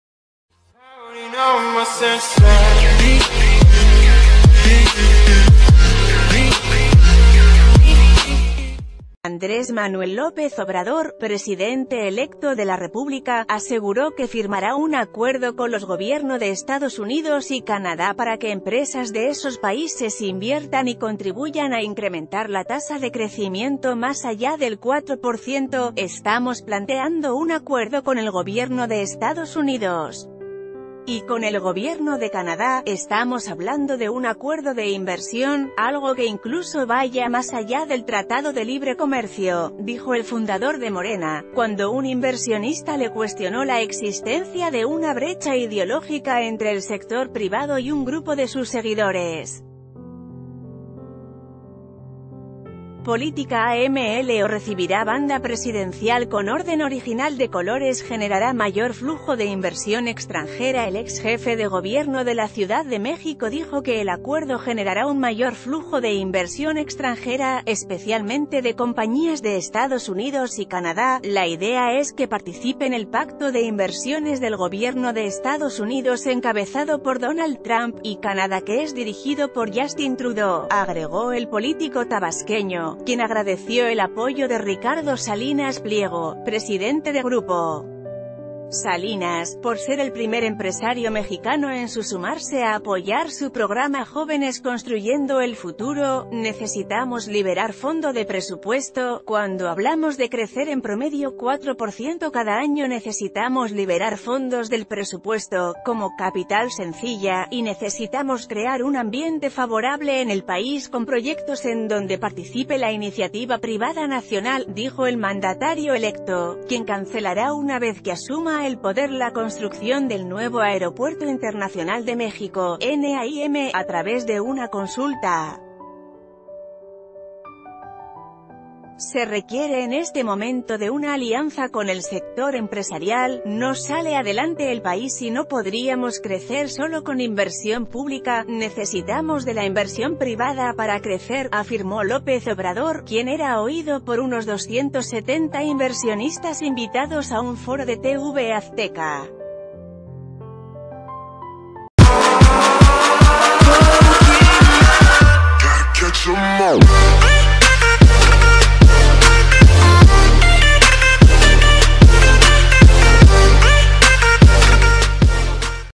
AMLO va por un acuerdo para atraer inversiones de EU y Canadá
Andrés Manuel López Obrador, presidente electo de la República, aseguró que firmará un acuerdo con los gobierno de Estados Unidos y Canadá para que empresas de esos países inviertan y contribuyan a incrementar la tasa de crecimiento más allá del 4 por ciento.“Estamos planteando un acuerdo con el gob...